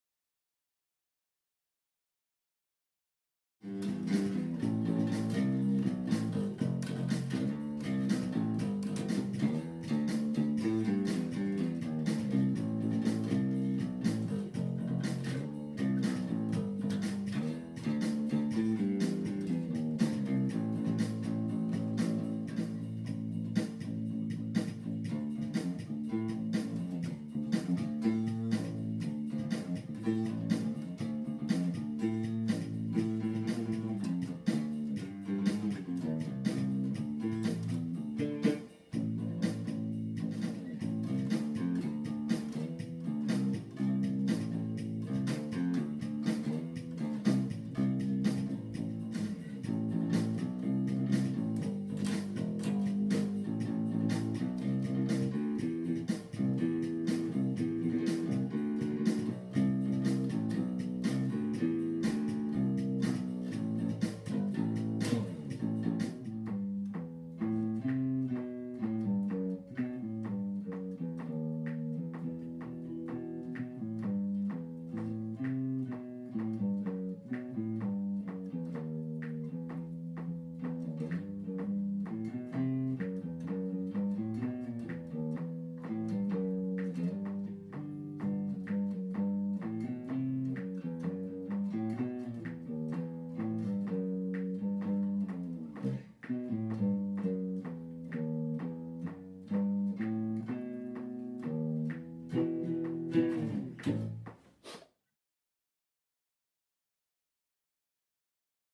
Bass Guitar Jam 2016
Jazzing at home with my Fender bass guitar..
Bass Guitar: Fender Precision Bass
Amplifier: None
Effects: None
...just plugged into my iMac home computer. Sound is awful!